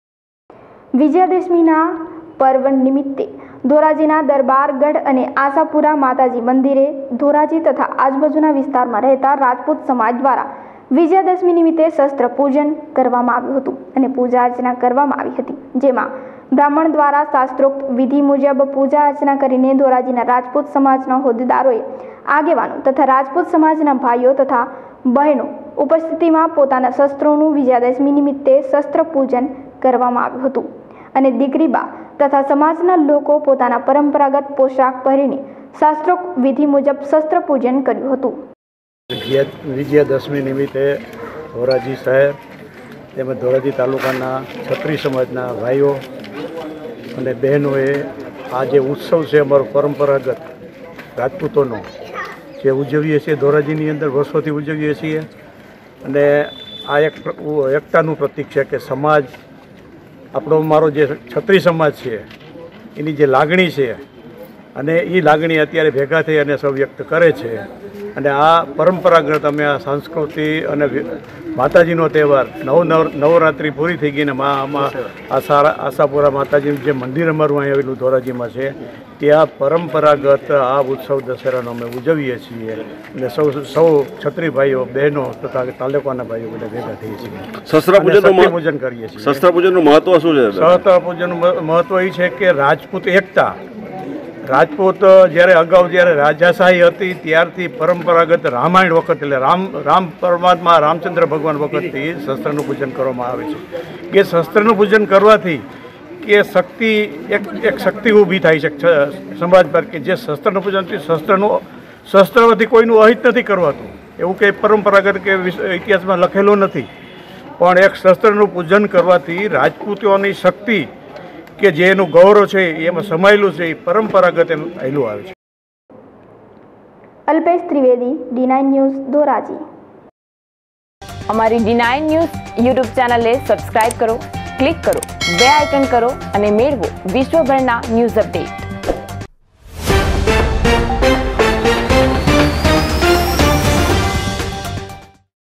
ધોરાજી માં વિજયાદશમીના પાવન પર્વ પર નિમિતે શસ્ત્રપૂજન
Title:  ધોરાજી માં વિજયાદશમીના  પાવન પર્વ પર નિમિતે શસ્ત્રપૂજન
Name of the Reporter :
Date & Time : 16/10/2021 12:00:00

Descriptions
વિજ્યાદશમી આ વિજ્યાદશમી નિમિત્તે આજરોજ ધોરાજી ના દરબાર ગઢ અને આશાપુરા માતાજી મંદિર મા ધોરાજી તથા આજુબાજુ ના વિસ્તાર મા રહેતા રાજપૂત સમાજ દ્વારા આજરોજ વિજ્યાદશમી નિમિત્તે શસ્ત્ર પુજન પુજા અર્ચના કરવામા આવેલ જેમા બ્રાહ્મણ દ્વારા શાસ્ત્રોક્ત વિધિ પુજા અર્ચના કરીને ધોરાજી ના રાજપૂત સમાજ ના હોદ્દેદારોએ આગેવાનો તથા રાજપૂત સમાજ ના ભાઈઓ તથા બહેનો ની ઉપસ્થિત મા પોતાના શસ્ત્રો નુ વિજ્યાદશમી નિમિત્તે શસ્ત્ર પુજન પુજા અર્ચના કરવામા આવેલ અને દિકરી બા તથા સમાજ ના લોકો પોતાના પરંપરાગત પોશાક પહેરી ને શાસ્ત્રોક્ત વિધી મુજબ શસ્ત્ર પુજન કર્યુ હતુ :